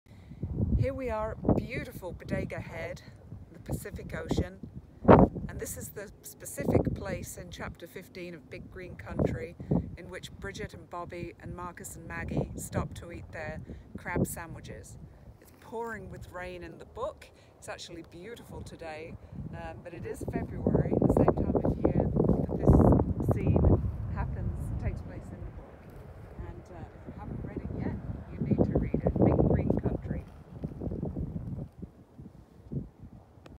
On the Road with Big Green Country — A Novel, By Frances Rivetti
Author Frances Rivetti with Big Green Country at the scene at Bodega Head at the Pacific Ocean where Bridget and Bobby, Marcus and Maggie eat fresh crab sandwiches at the start of their unforgettable road trip north along the Sonoma Coast.